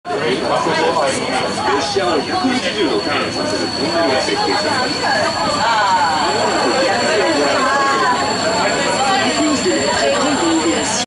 フロム鉄道から眺める景色
2023年5月22日、ノルウェーのベルゲン鉄道の車窓から眺める景色です。